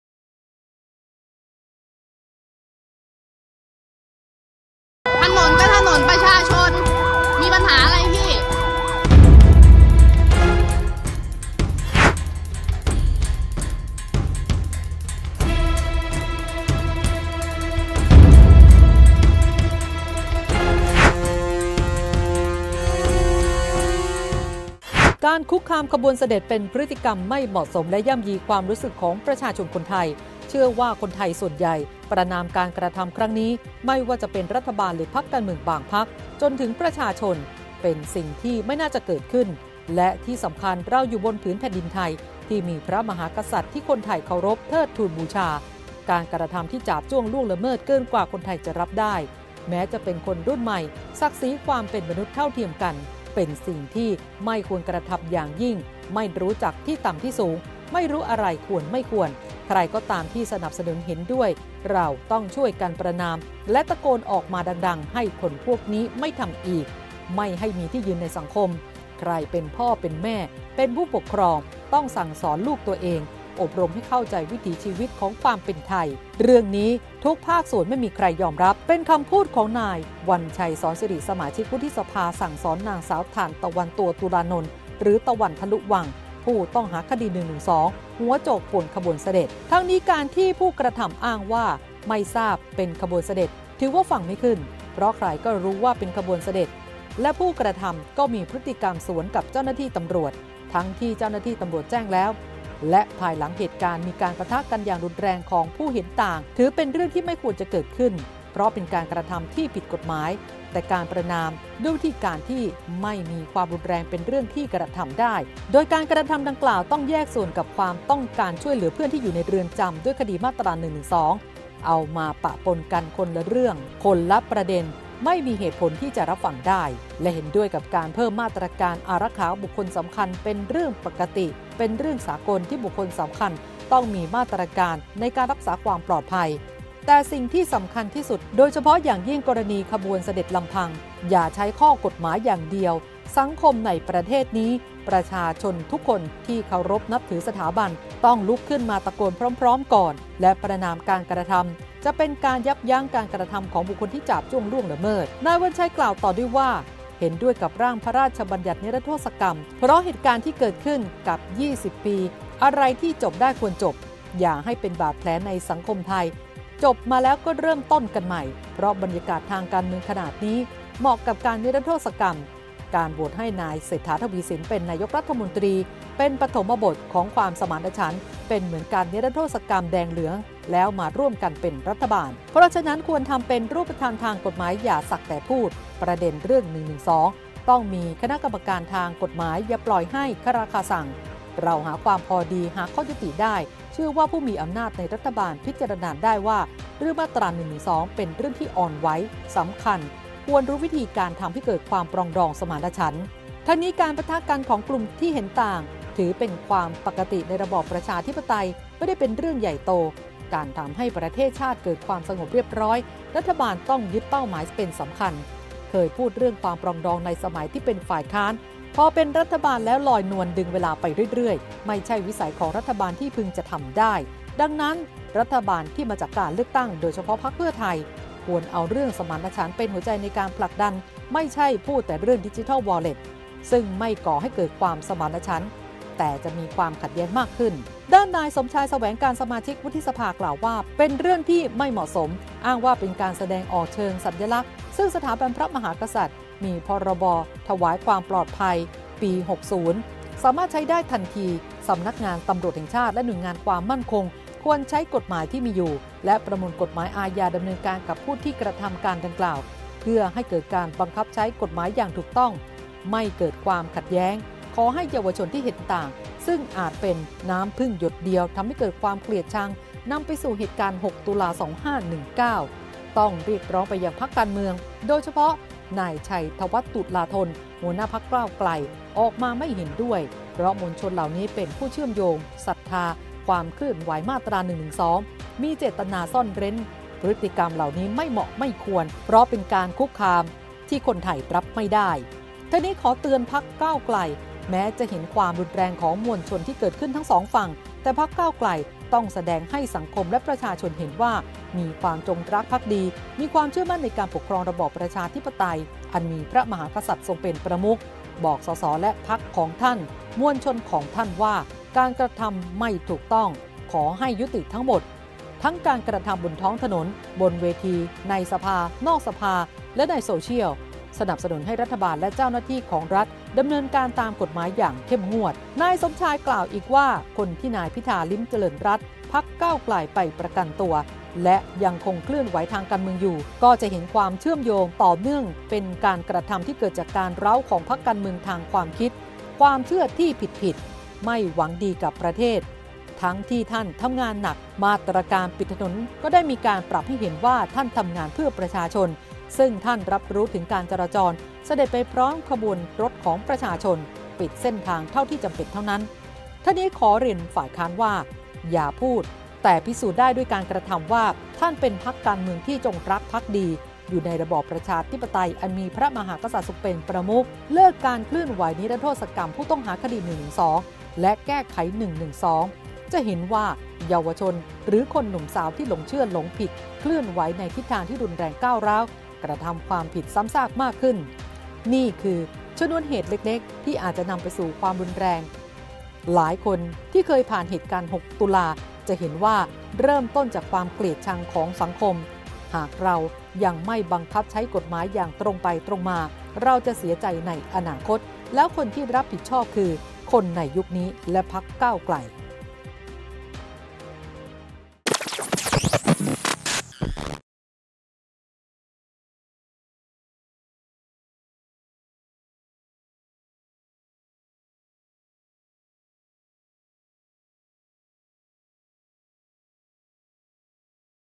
ซัด "ก้าวไกล" ปั่นหัวสาวก สั่งสอนหัวโจก ไม่รู้จักที่ต่ำที่สูง | TOPUPDATE
"ส.ว."รุมซัด"ก้าวไกล"ปั่นหัวสาวก สั่งสอนหัวโจกป่วน "ขบวนเสด็จฯ"ไม่รู้จักที่ต่ำที่สูง จี้ยกเครื่องทีมอารักขาราชบัลลังก์